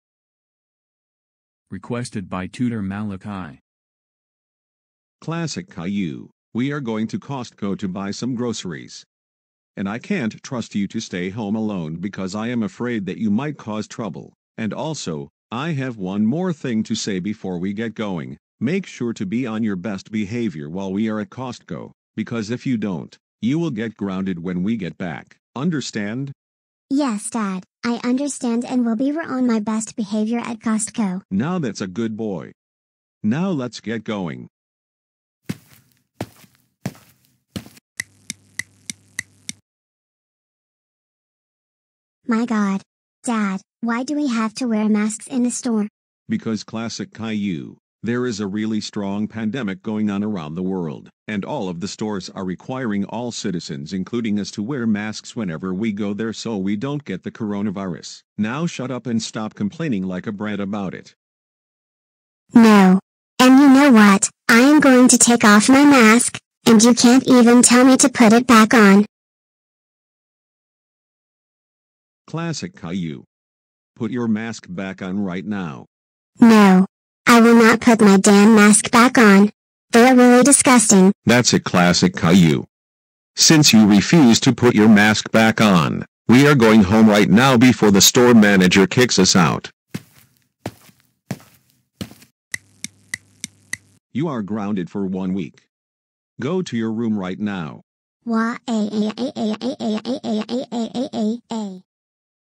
Classic Caillou Refuses To Wear A Mask At A Store/Grounded
Re-uploaded because I made an expression error on Boris in the final scene of the video.